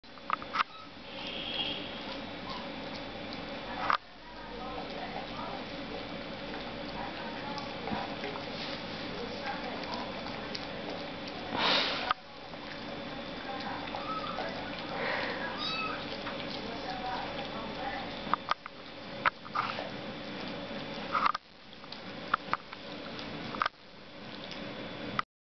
פולק וצ'ומפי
צ'ומפי מענגת את פולק בת ה-6 שבועות